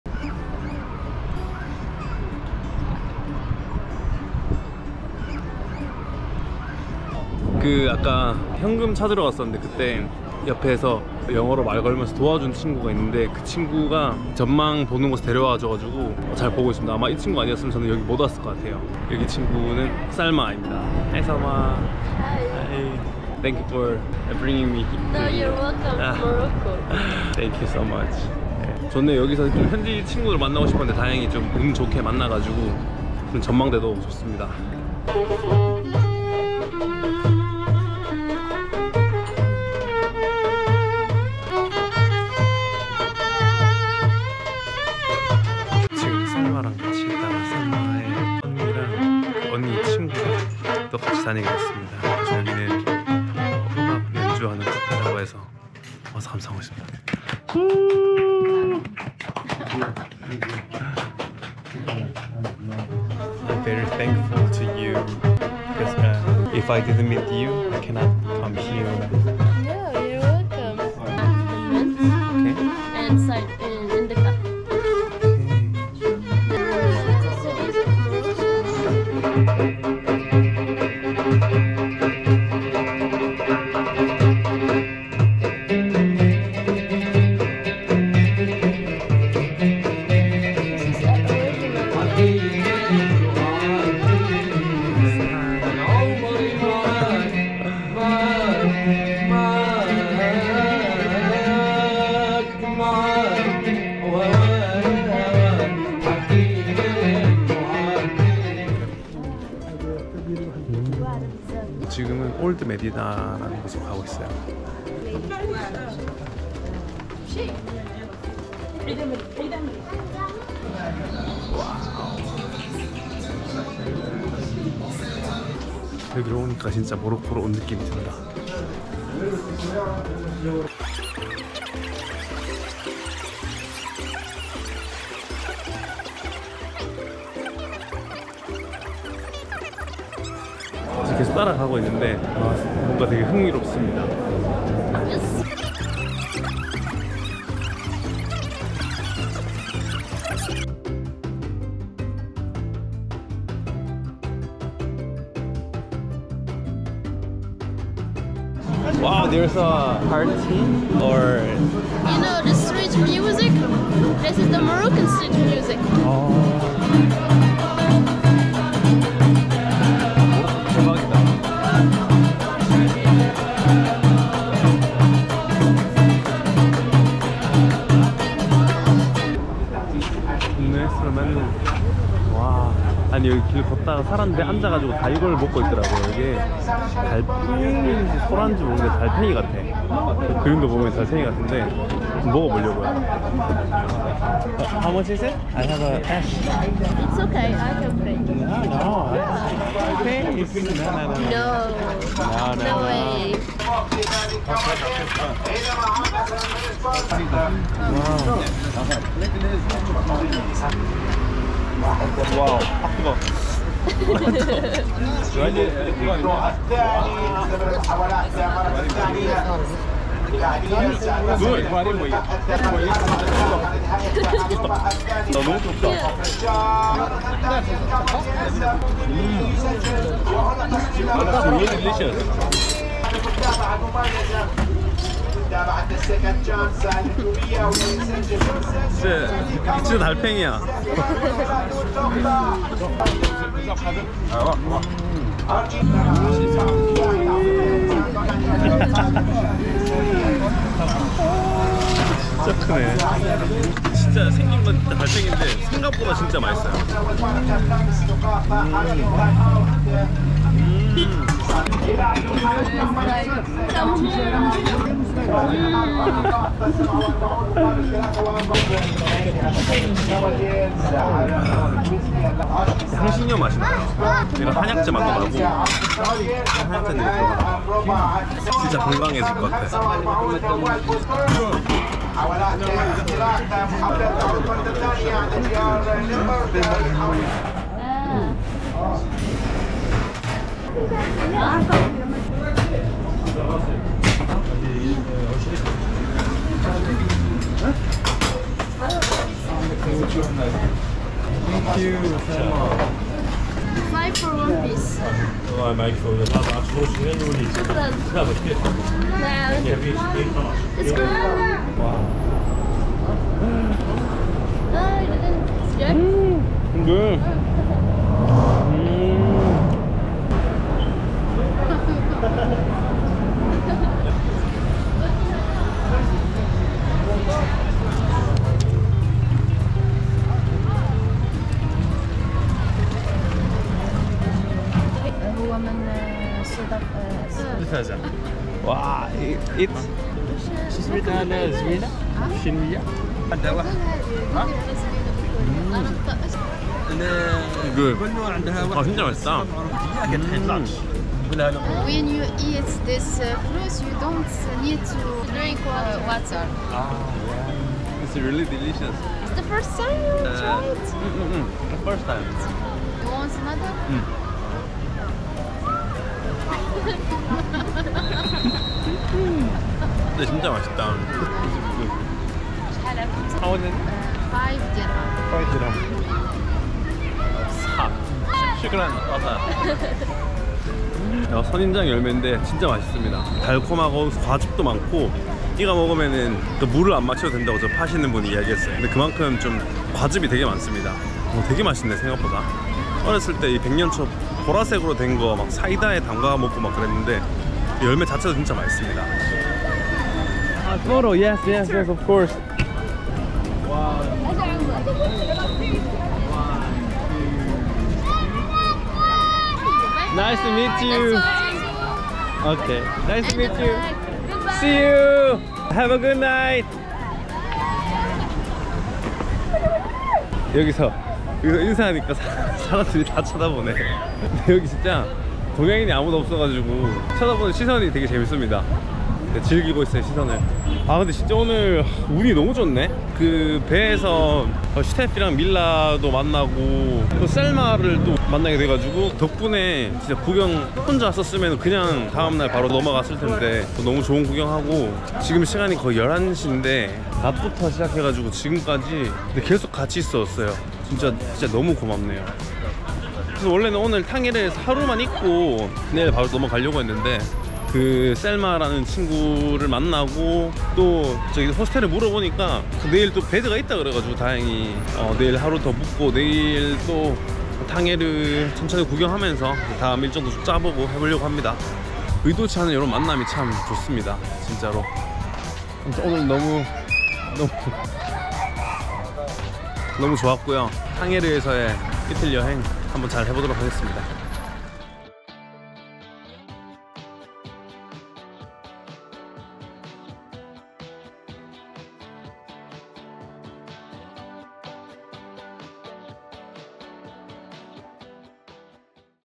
모로코 친구와 탕헤르 구경하고 길거리음식 먹어보기 - 【모로코 2】
모로코사람 친구를 사귀게 되어서 덕분에 재밌게 탕헤르 구경 했네요!
달팽이 존맛!

Film : Gopro Hero 10 Black, i phone 12pro
Edit : Dell Inspriron 15 7000, Premiere pro 2022